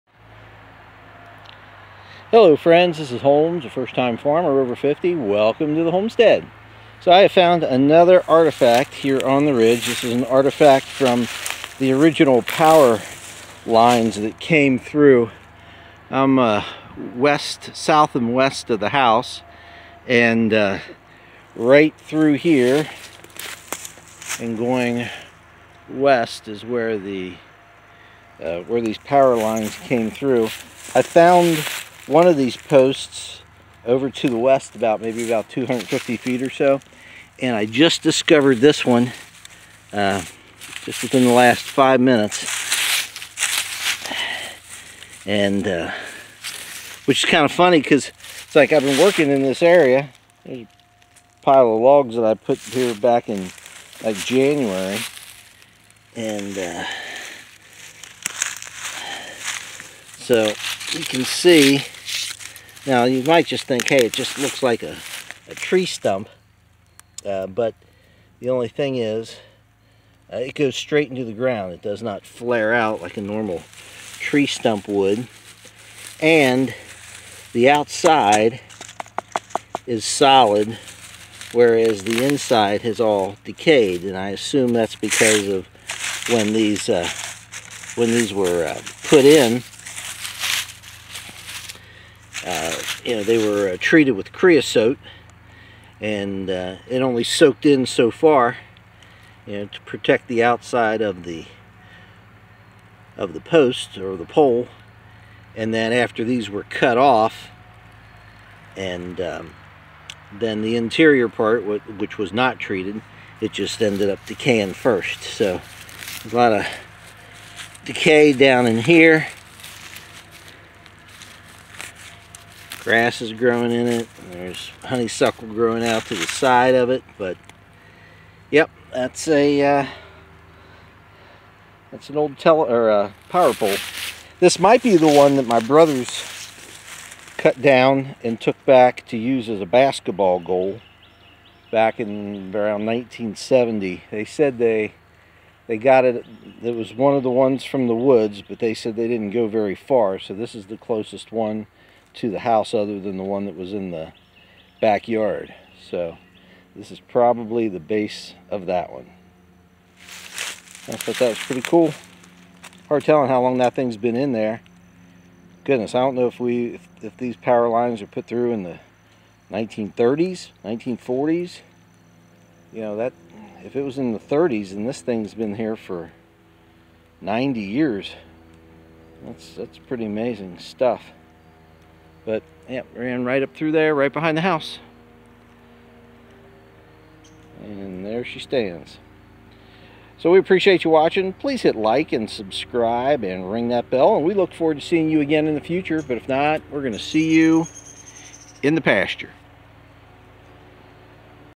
2024 10 11 Did I Find A 90 Year Old Artifact?
While working in the woods I found an artifact from the power lines which used to run across the Ridge. Not sure when they were put in but I do know they were removed in the 1960s. They have been gone for 60 years and they may have been around for 90 years!